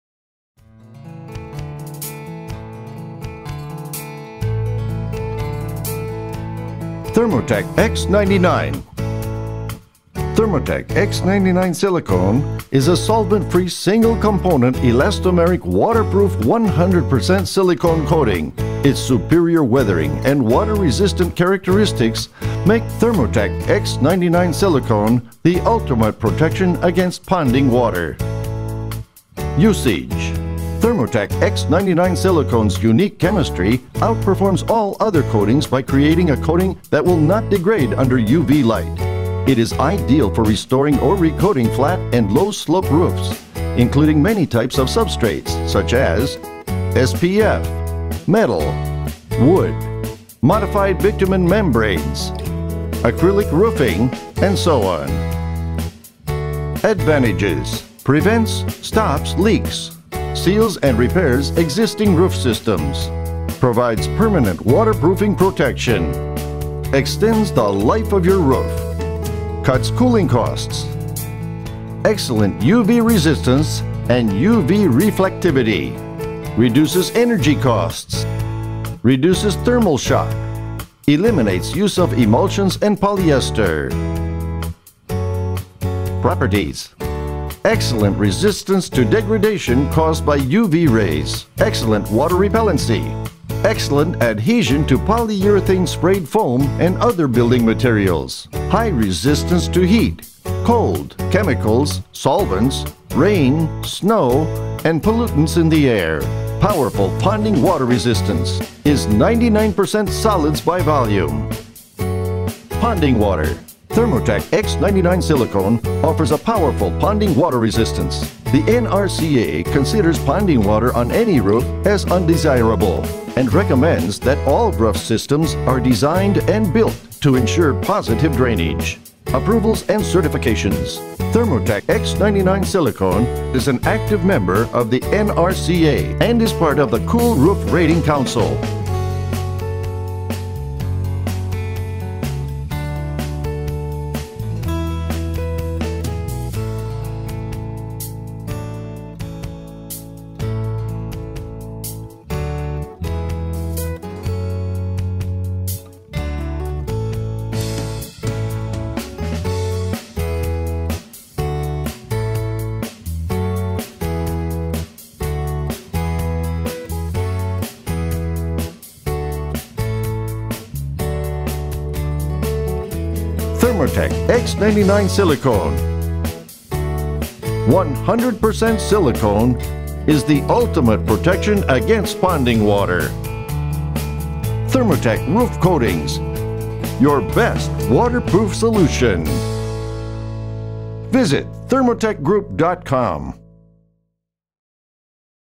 Thermotek X99 SILICONE (Short Version)
Thermotek X99 SILICONE outperforms all other coatings by creating a seamless membrane that will not degrade under UV light. Thermotek X99 SILICONE, ultimate ponding water.